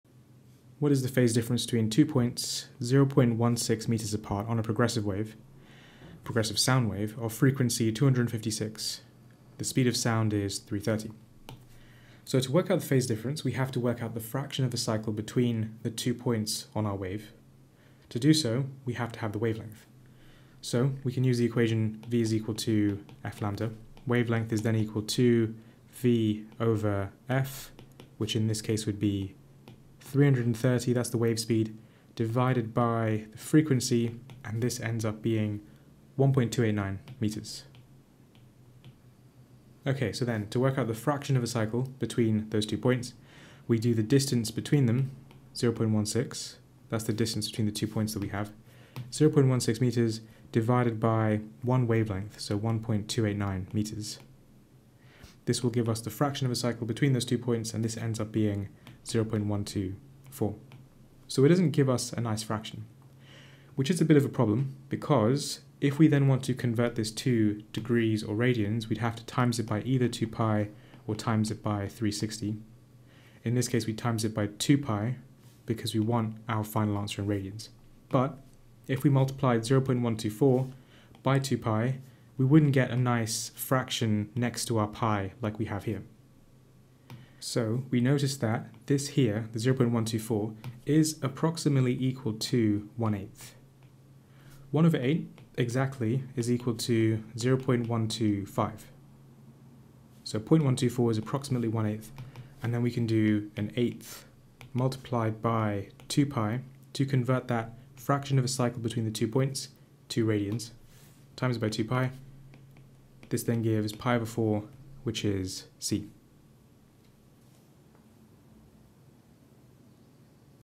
Waves (Path & Phase Difference) Exam Questions - A Level Physics AQA, Edexcel, OCR A (level 2)
Exam/Exam style questions in order of increasing difficulty. Give the questions a go and check out the videos if you get stuck!